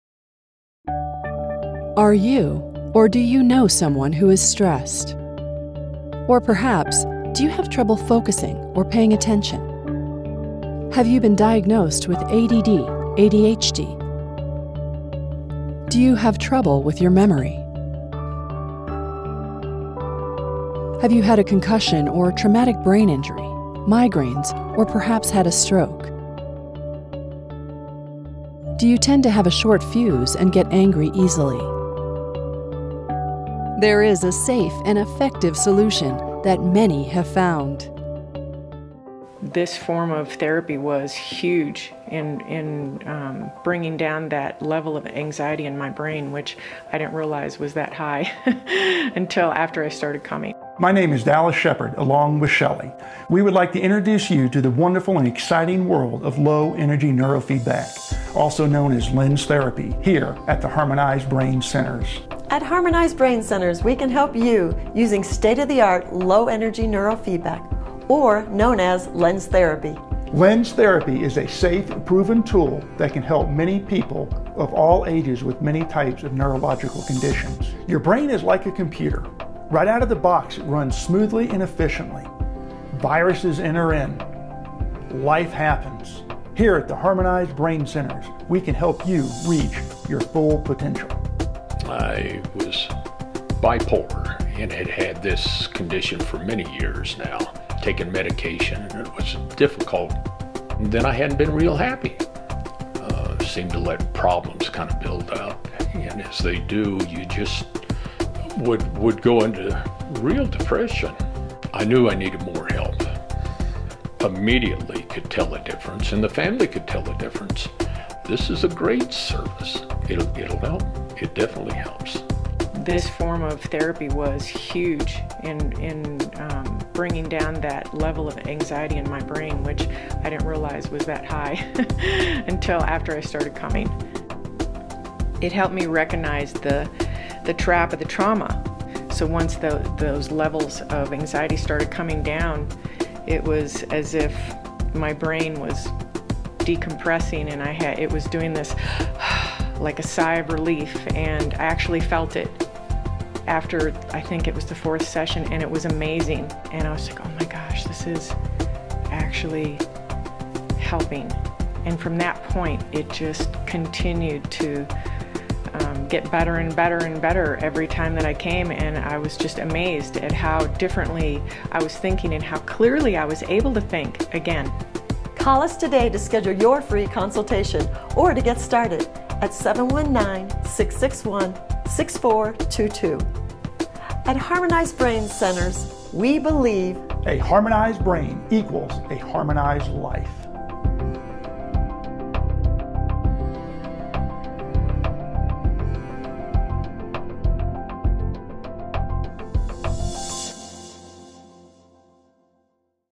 Harmonized Brain Centers
At the Harmonized Brain Centers we have a passion to help people with neurological conditions improve their lives by using safe, effective and efficient modalities to help your brain self regulate and perform better.

Are you or do you know someone who is
stressed or perhaps do you have trouble
focusing or paying attention?

Have you been diagnosed with a DD ADHD do you
have trouble with your memory have you
had a concussion or traumatic brain,
injury migraines or perhaps had a stroke?

Do you tend to have a short fuse and get
angry easily?

There is a safe and
effective solution that many have found.

This form of therapy was huge in
bringing down that level of anxiety in
my brain which I didn't realize was that
high until after I started coming.

My name is Dallas Shepard along with
Shelly, we would like to introduce you
to the wonderful and exciting world of
Low Energy Neurofeedback also known as
LENS therapy here at the Harmonized
Brain Centers.

At the Harmonized Brain Centers we can help
you using state-of-the-art low-energy
neurofeedback or known as lens therapy
lens therapy.  It is a safe proven tool that
can help many people of all ages with
many types of neurological conditions.

Your brain is like a computer.  Right out
of the box it will run smoothly and efficiently.
Viruses enter, life happens and our brains slow.
Here at the Harmonized Brain Centers we can help
you reach your full potential.

(Alan)
I was bipolar and I had this condition for
many years.  I have been taking medication but it was
difficult. I hadn't been real happy as I
seem to let problems kind of build up
and as they do you just would go into
real depression I knew I needed more
help.  Immediately I could tell the difference
and my family could tell the difference.
This is a great service.

(Barb)
It definitely helps. This form of therapy
was huge in bringing down that level of
anxiety and my brain which I didn't
realize was that high until after I
started coming. It helped me recognize
the trap of the trauma so once those
levels of anxiety started coming down, it
was as if my brain was decompressing and
I had this like a sigh of
relief and I actually felt it. After I
think it was the fourth session it
was amazing and I was like oh my gosh
this is actually helping and from that
point it just continued to get better
and better and better every time.   I
came and I was just amazed at how
differently I was thinking and how
clearly I was able to think again.

We Believea Harmonized Brain equals a Harmonized
Life